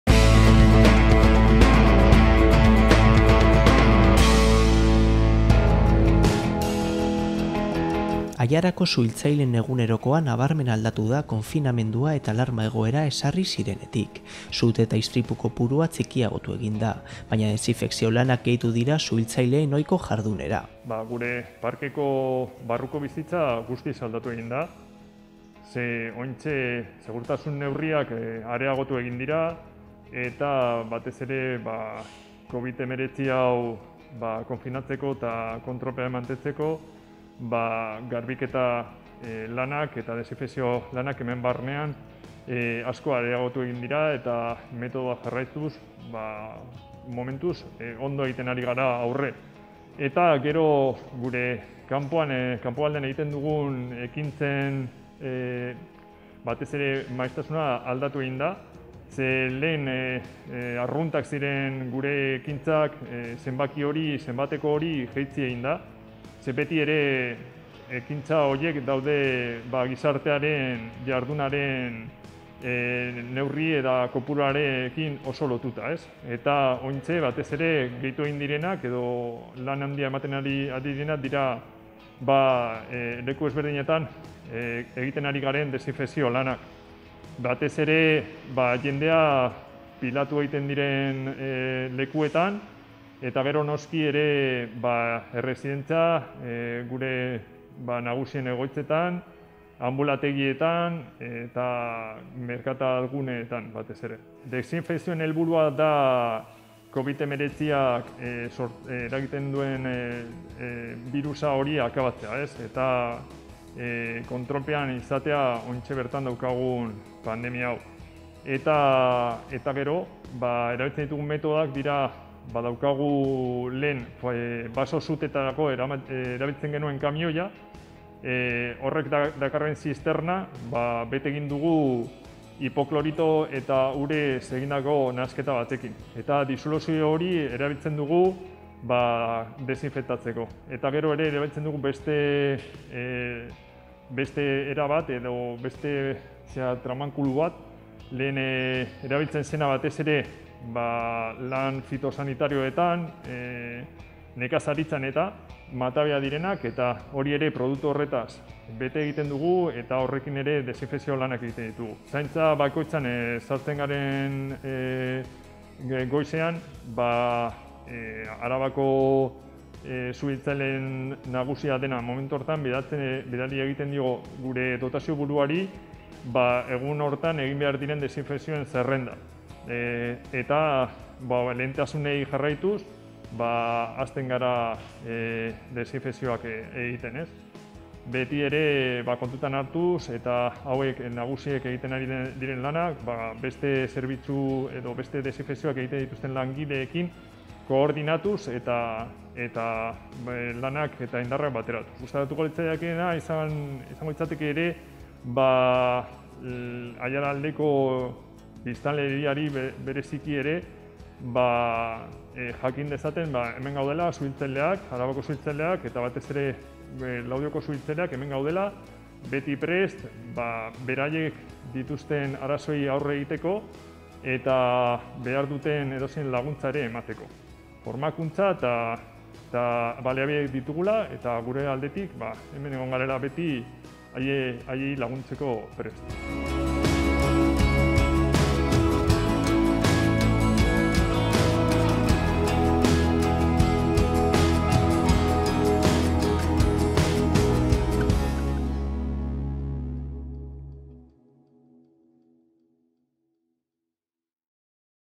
Nola egiten dituzte suhiltzaileek desinfekzio lanak?
Aiarako Suhiltzaileen egunerokoa nabarmen aldatu da konfinamendua eta alarma egoera ezarri zirenetik.  Sute eta istripu kopuruak txikiagotu egin dira, baina desinfekzio lanak gehitu dira suhiltzaileek ohiko jardunera.